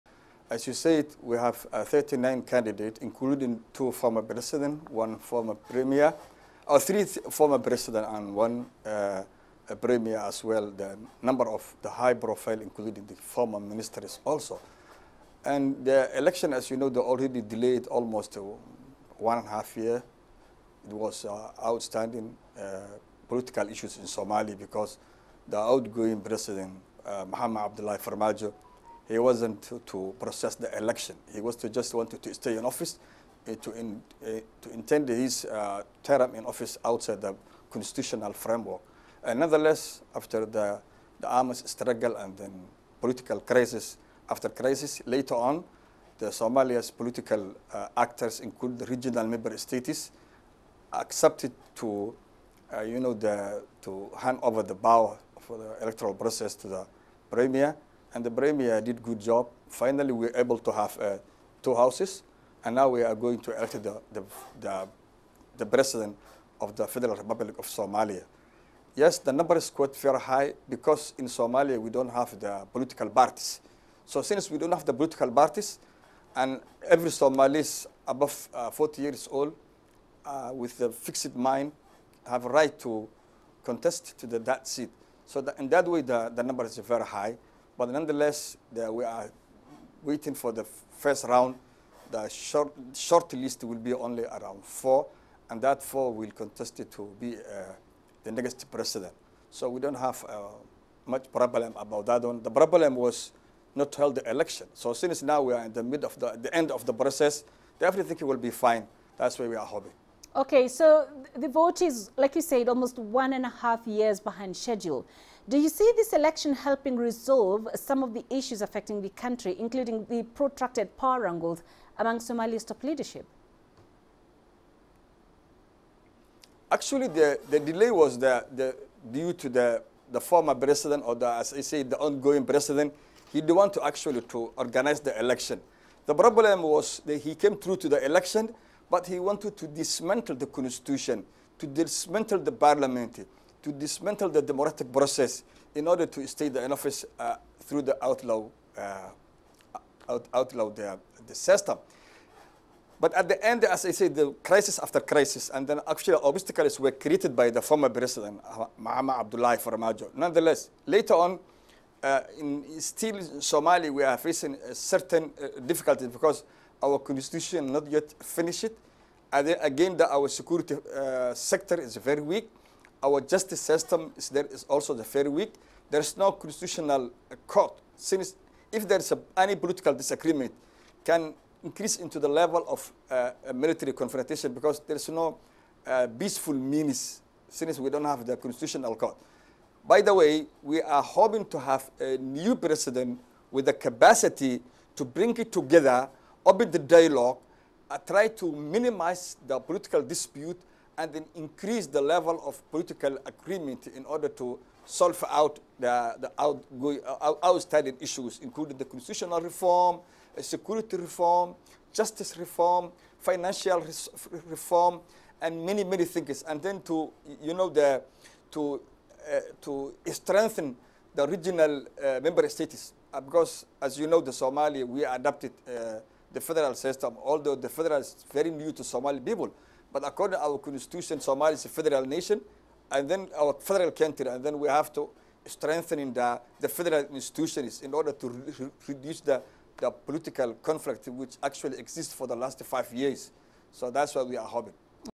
A record 39 candidates are running for Somalia's presidential election
A record 39 candidates have registered for the Presidential race in Somalia that is taking place on Sunday, May 15. More than 300 MPs will be in charge of deciding who will be the next leader of the Easternmost African nation.

Current president Mohamed Abdullahi Mohamed or Farmaajo is running again after an attempt to extend his four year first term in 2021.

Former Foreign Minister, Fawzia Yusuf Adam is the only female candidate.